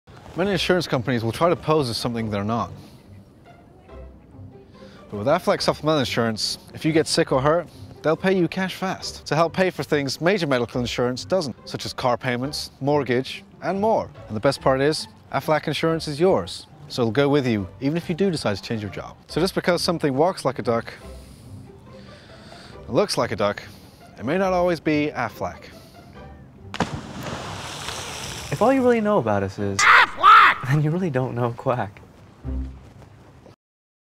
AFLAC Commercial contest
My entry for the recent 2009 Aflac Commercial Contest. We did make the top 10 but didn't win. We did get a care package!